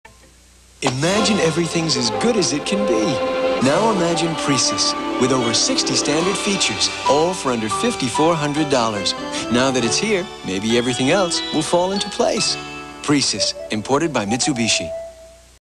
1988 Mitsubishi Precis "Imagine it is as good as it can be" TV Commercial
This commercial aired September 18th, 1988. It was run during the broadcast of "ABC Closeup: Beyond the Shuttle" on WTVD Channel 11 in Durham/Raleigh/Fayetteville, North Carolina.